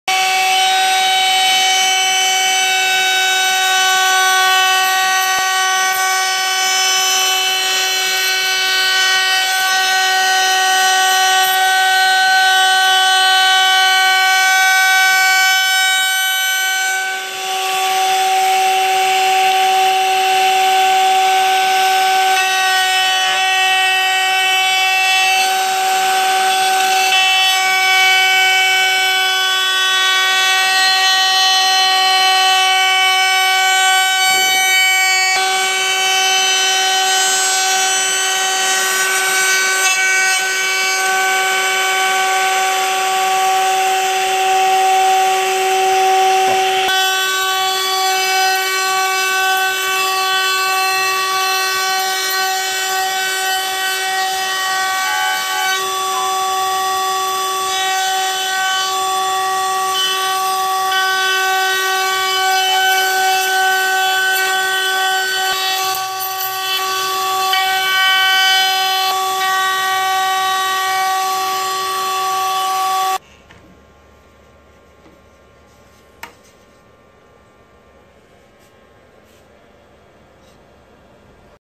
Resonator neck shaping
Using a pin router with a custom bit to do the basic neck shaping of Honduras mahogany necks for my biscuit bridge resonators